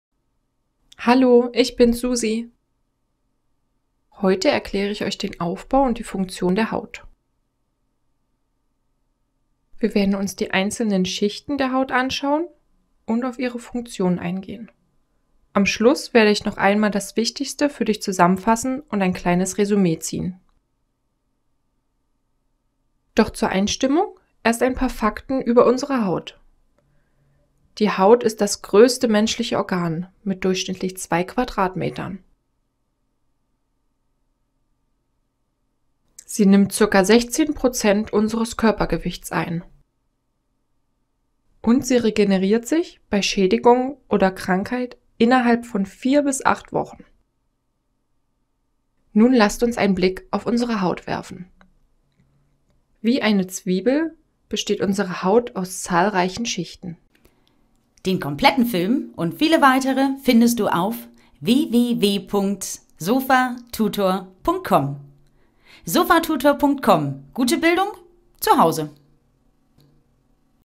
Aufbau und Funktion der Haut | Biologie | Humanbiologie
Die Haut ist das größte Organ des menschlichen Körpers. Lerne in diesem Video deren Aufbau und die einzelnen Funktionen. Die Haut ist in die Oberhaut, Lederhaut und Unterhaut gegliedert. Diese Schichten werden dir in dem Video noch detaillierter vorgestellt. Außerdem lernst du interessante Daten über die Haut. So erfährst du zum Beispiel wie groß deine Hautoberfläche ist und wie schwer die Haut im Vergleich zu deinem Körpergewicht ist. Du erfährst zusätzlich, wann eine Narbe bei einer Verletzung zurückbleibt und wann nicht.

Dieses Video gehört zu: Biologie | Humanbiologie | Anatomie